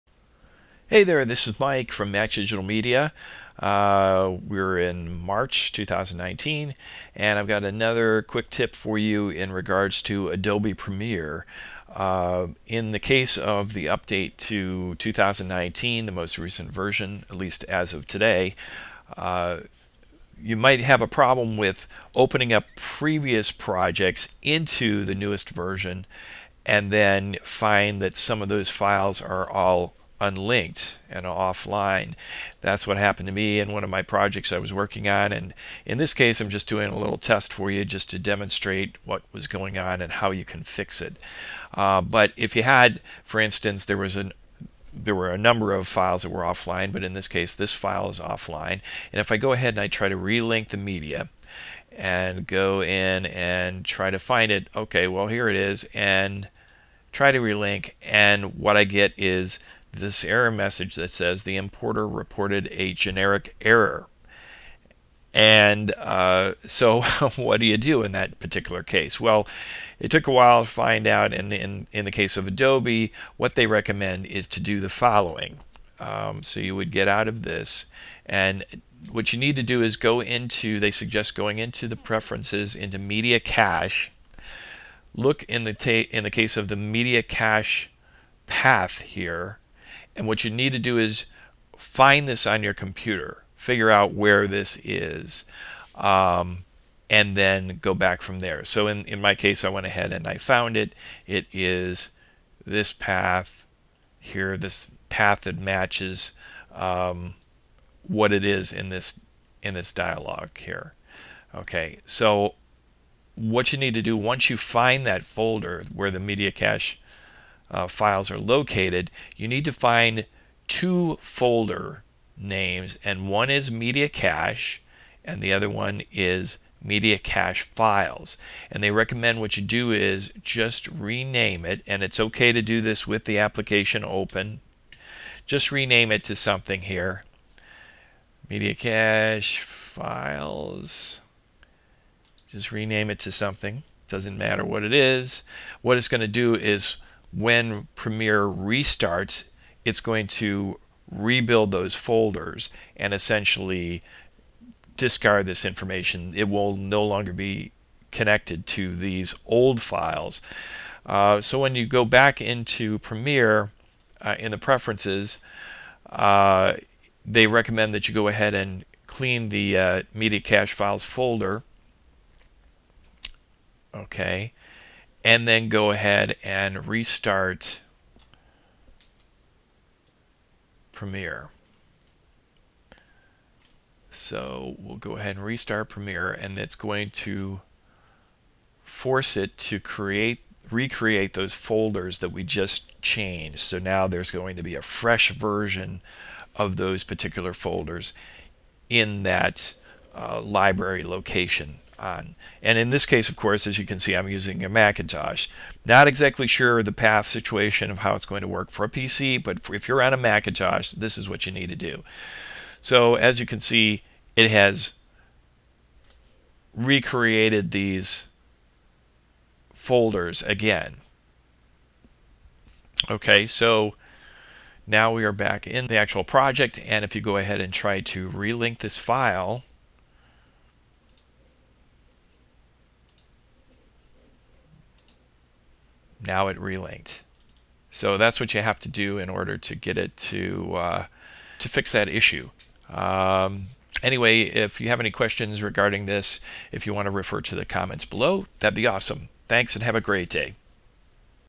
T006 - Adobe Premiere Generic Error Relinking fix
Method for fixing an issue with files that have become unlinked in Adobe Premiere and show a "generic error" when attempting to reimport.

I was running Mac OS Sierra when I did the demo.  This problem seems to have come up before, so not just with the new 2019 version of Premiere.

The previous renamed versions of the "Media Cache" and "Media Cache Files" folders that I show in the example can later be removed from your system.  These are the one's with my _xxxx addition.

On the Macintosh, be sure you are accessing this correct Library folder for the default Media Cache location:    /Users/yourUserName/Library/Application Support/Adobe/Common/

There are multiple Library folders on your Mac.  The one you need to locate is the Library folder associated with your primary user account (your Home folder, the one with your username, located in the Users folder of your startup drive).  This file is typically hidden, so here's how to find it.

You can access the correct user Library folder by doing the following:
1) Navigate to the Finder level on your Mac.

2) Select the "Go" pulldown menu.

3) With this "Go" menu selected, hold down the OPTION key and your hidden Users Library will appear in the list.

4) Select and open this Library folder.

From there you will be able to navigate to the correct place to find your Media Cache and Media Cache Files folders, as described in my video.

.../Library/Application Support/Adobe/Common/

The key is that you use the OPTION key to reveal the hidden User Library folder in the Finder. That is the one you need to use to locate the correct files.